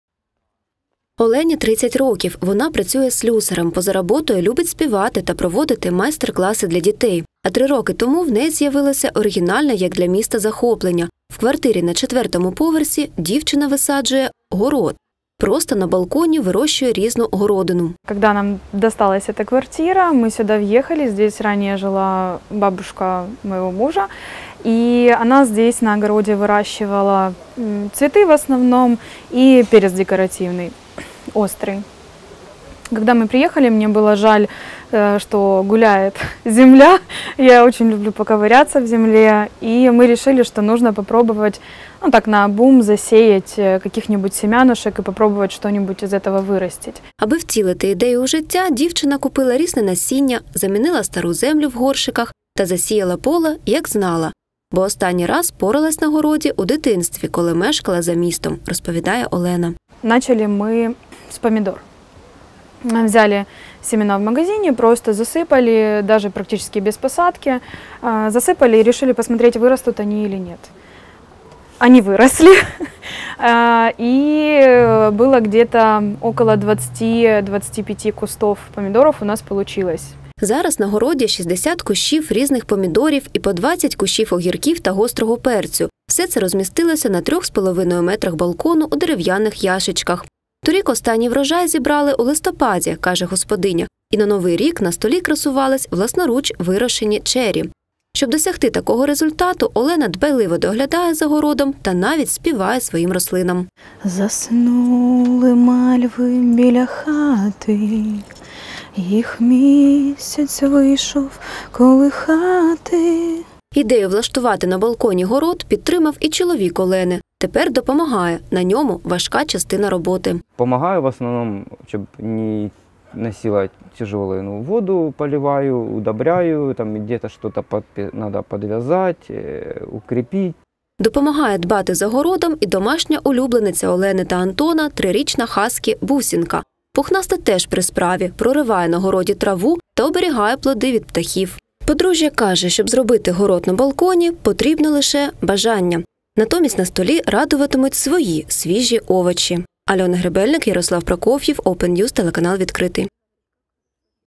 Черрі, огірки та гострий перець. У Дніпрі дівчина вирощує овочі та зелень прямо на балконі.
Черрі, огірки та гострий перець. І все це на власному балконі. У Дніпрі дівчина вирощує овочі та зелень прямо у квартирі. А доглядати за господарством їй допомагають чоловік та собака.